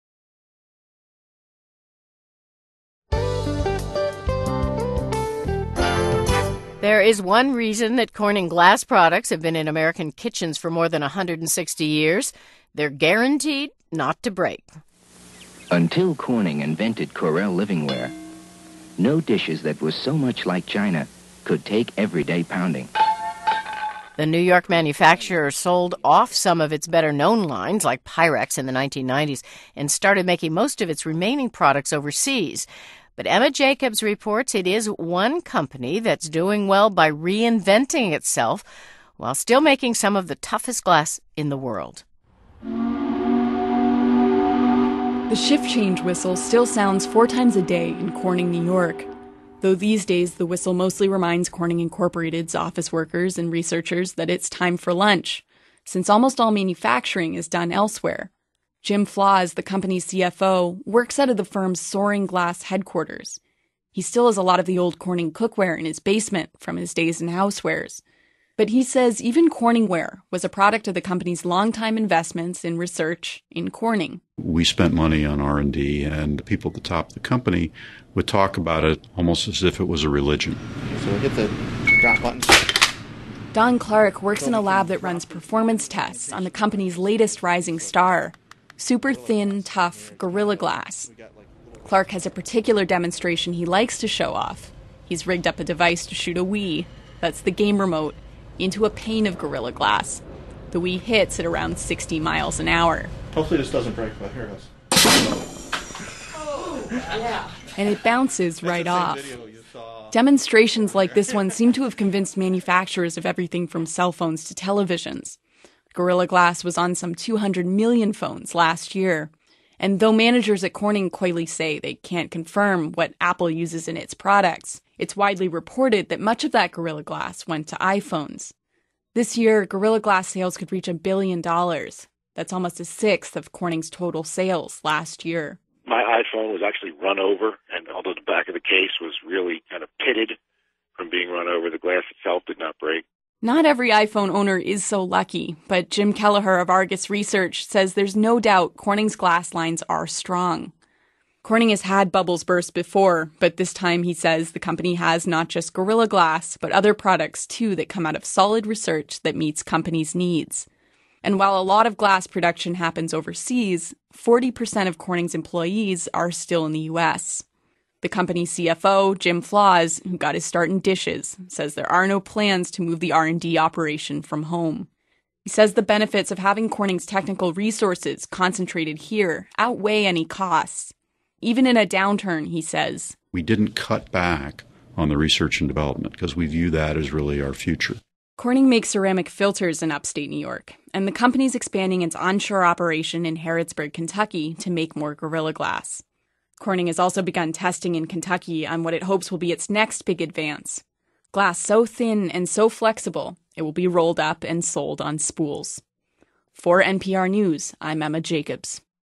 Gorilla Glass - NPR interview with Corning CFO Jim FLaws
NPR Gorilla Glass interview with Jim Flaws, Corning Inc. CFO in March 2011